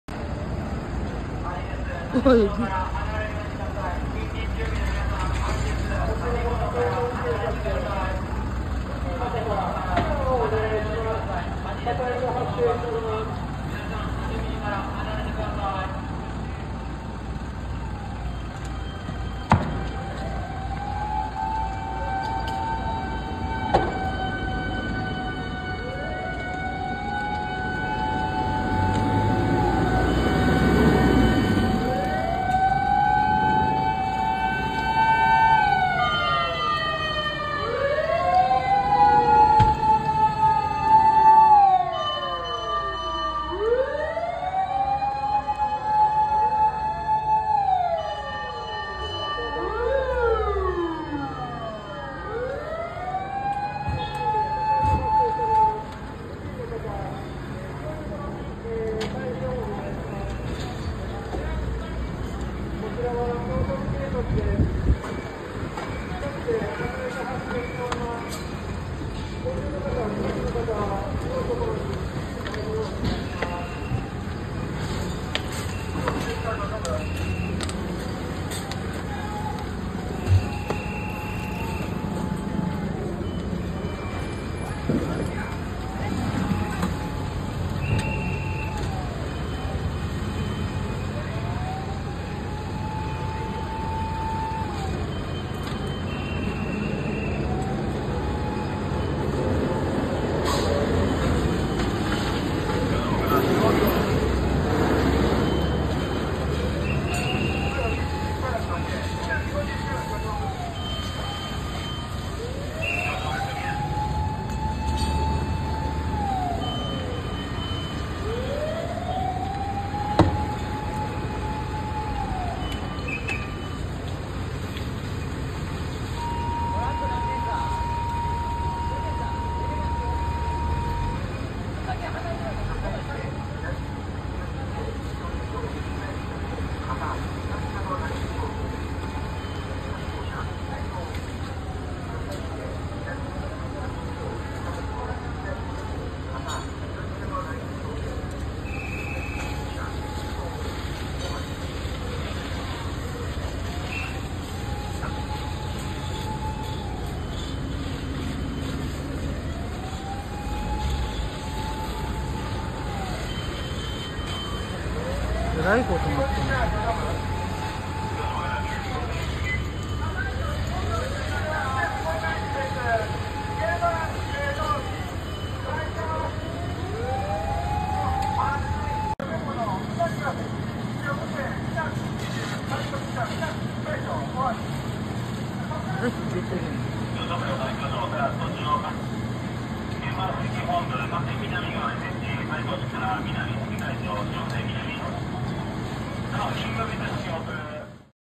京都市南区の3階建て建物で火災、60代女性が心肺停止 Fire breaks out in a three-story building in Minami Ward, Kyoto City
【読者提供動画】2024年5月17日の午前11時半ごろ、京都市南区河原町通九条下ルで、通行人から「建物から煙が見える」と119番があった。京都市消防局によると、3階建ての建物から出火した。建物内にいたとみられる60代の女性を救出したが、心肺停止の状態という。

[Video provided by readers] At around 11:30 a.m. on May 17, 2024, a passerby called 119 on Kujo Sagaru, Kawaramachi-dori, Minami-ku, Kyoto City, saying, ''Smoke can be seen from the building.'' According to the Kyoto City Fire Department, the fire broke out in a three-story building. A woman in her 60s who was believed to have been inside the building was rescued, but she was said to be in cardiac arrest.